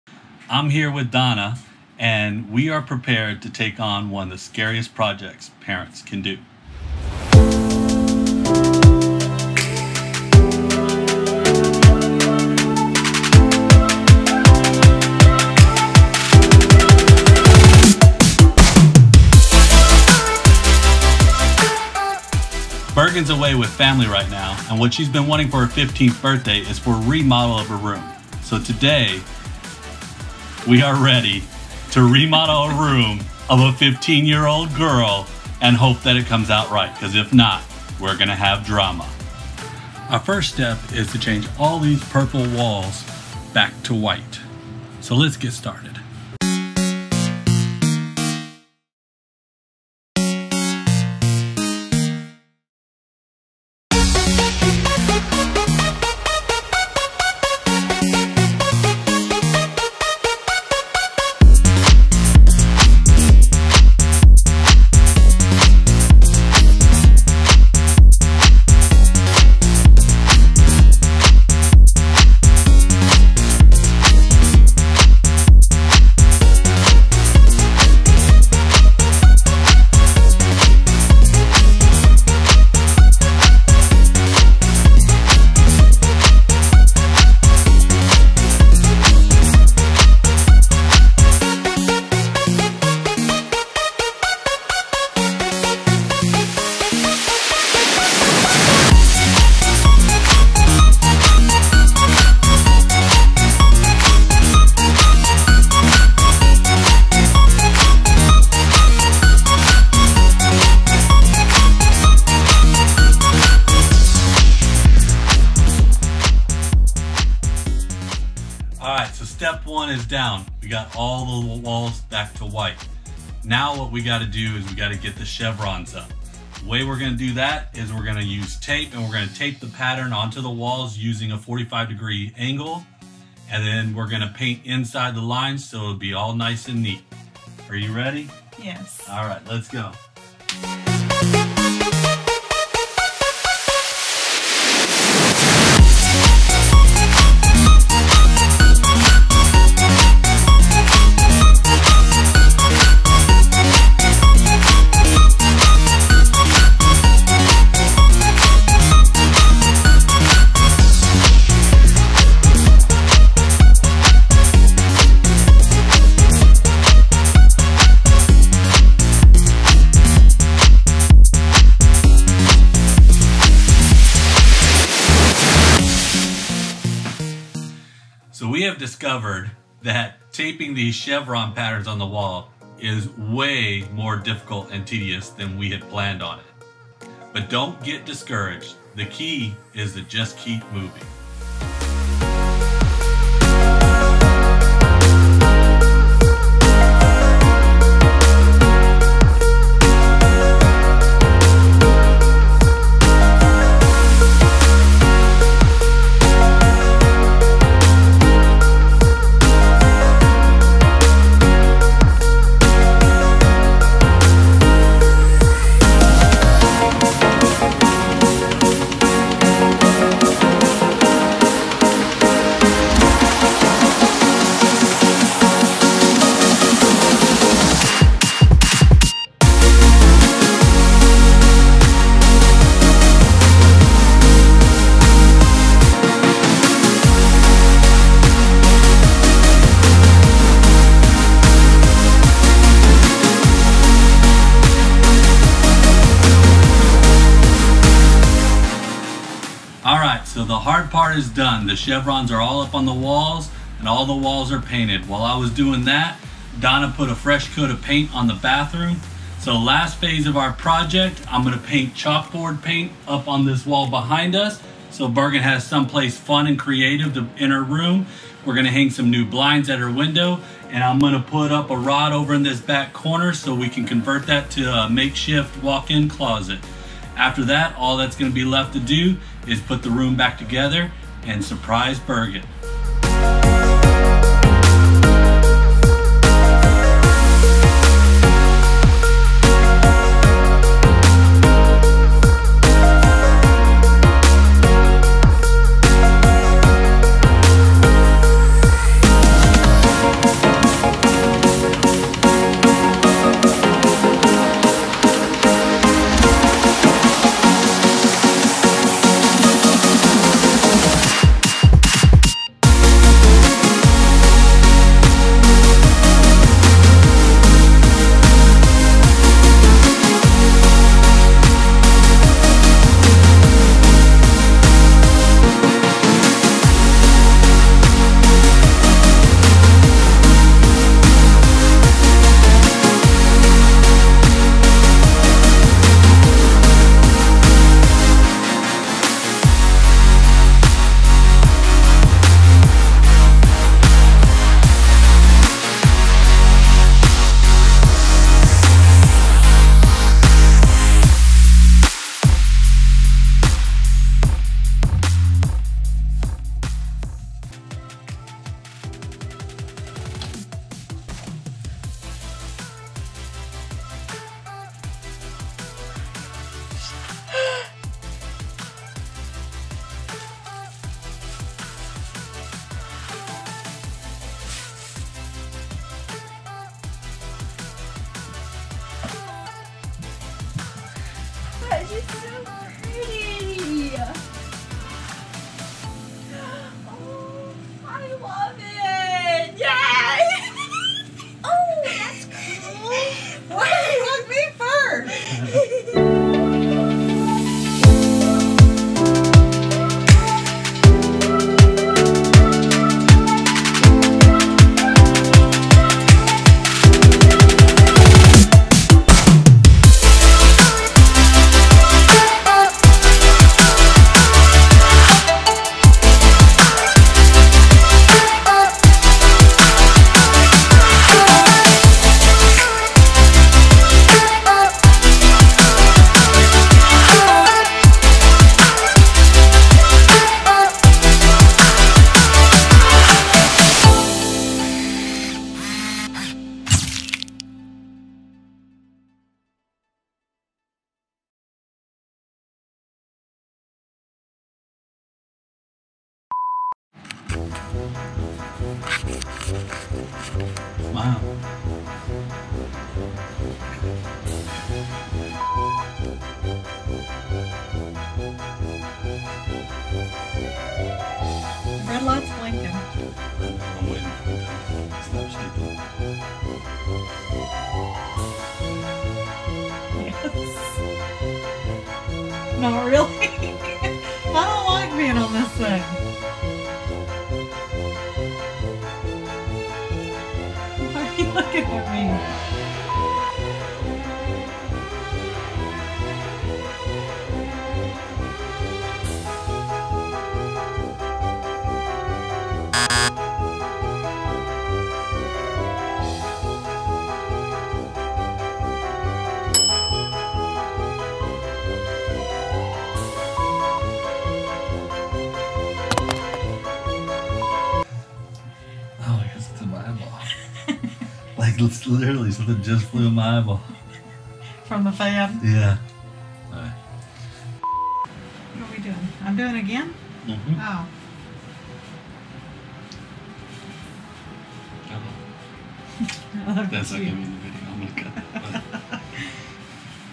Teenage bedroom makeover - Chevron painted walls!
Watch Jeremy and Donna's remodel of Bergen's room as a surprise 15th birthday gift!

Shot on goprohero7black